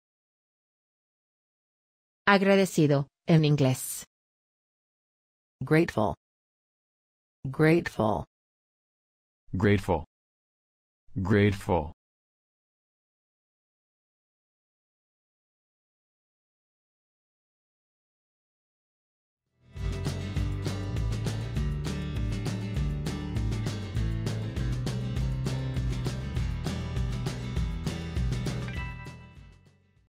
Cómo se dice agradecido en inglés
¿Cómo se dice correctamente agradecido en inglés? Escuche con nosotros.
¿Cuál es la traducción correcta de la palabra agradecido del español al inglés? Habla como los americanos. Además, ¿cómo escribir correctamente la palabra grateful?

¡Gracias!